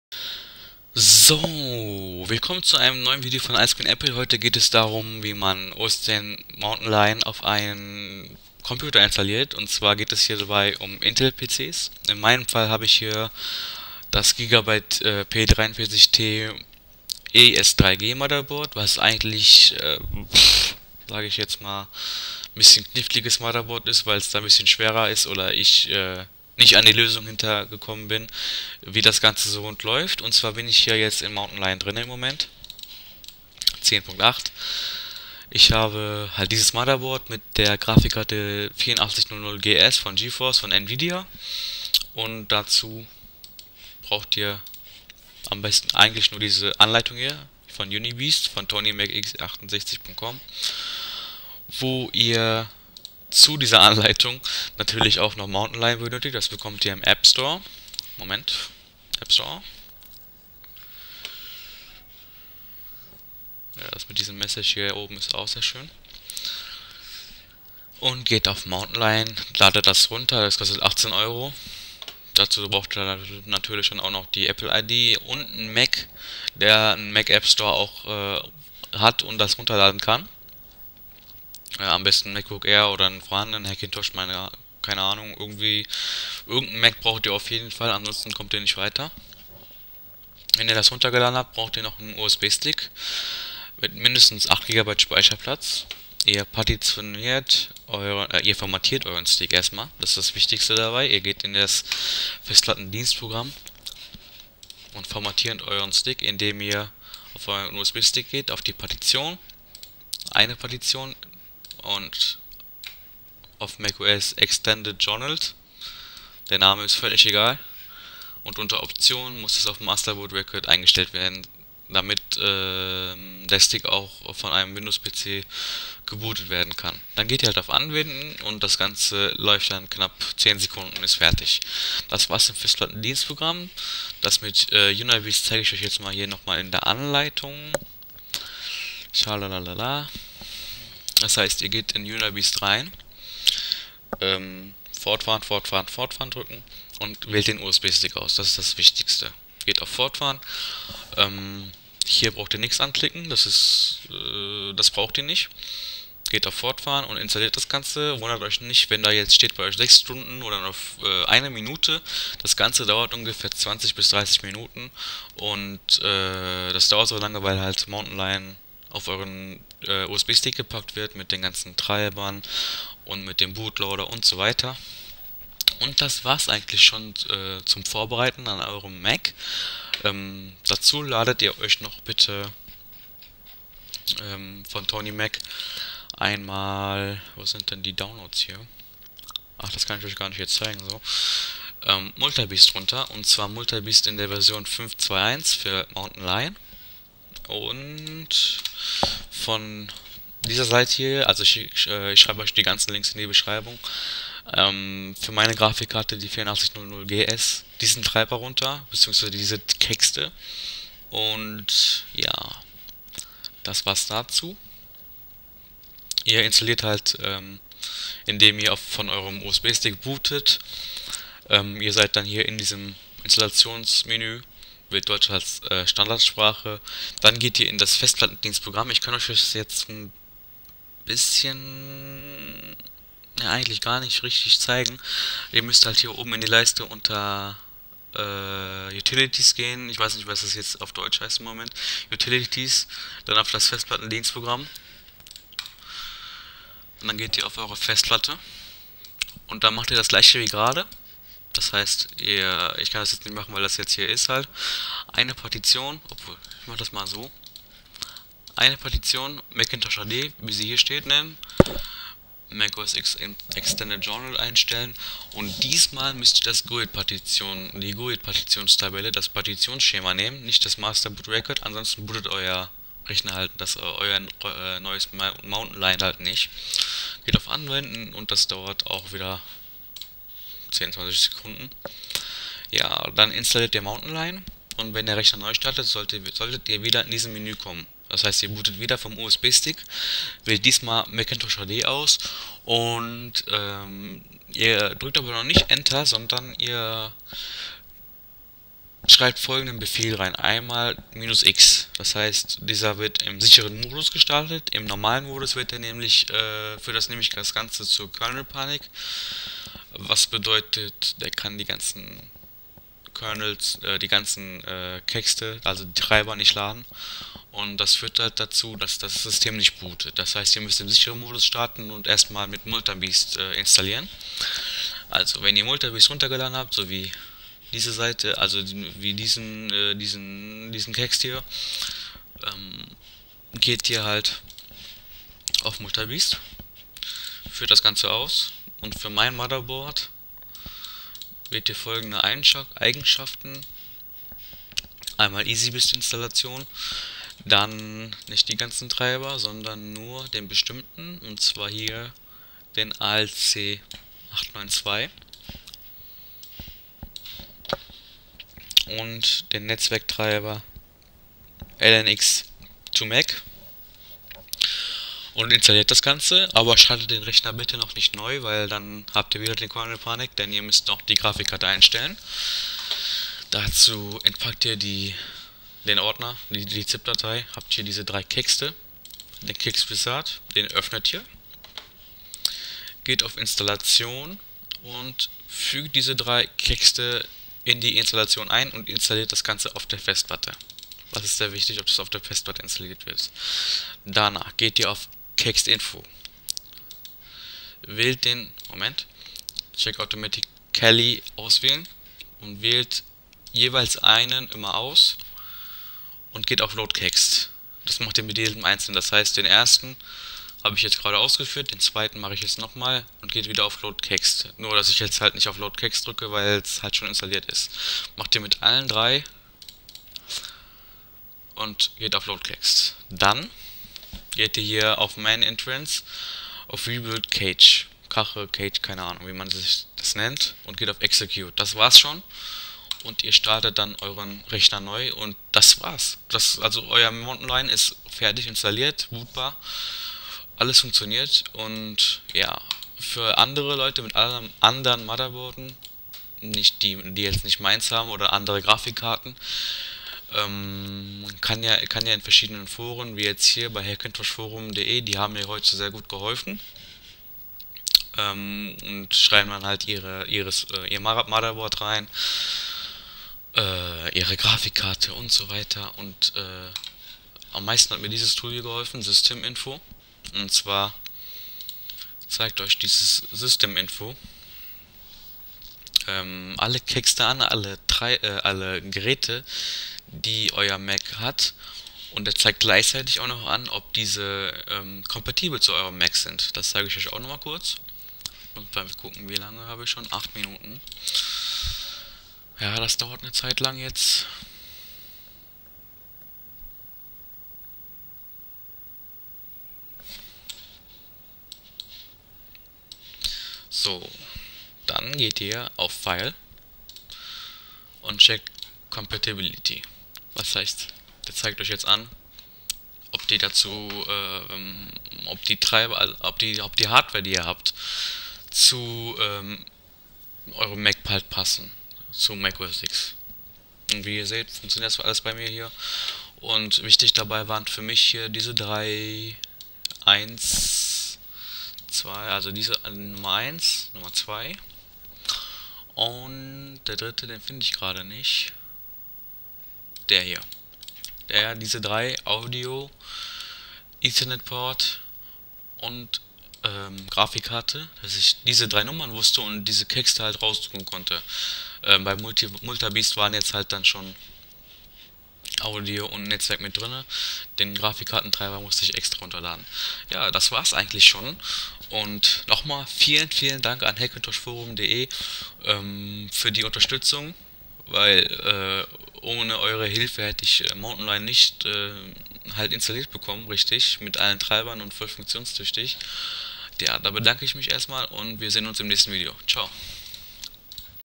Installation von OS X Mountain Lion 10.8 auf einem PC - Hackintosh
Hey Leute, heute installiere ich auf meinem GA-P43T-ES3G Motherboard bzw. ich zeige wie es funktioniert.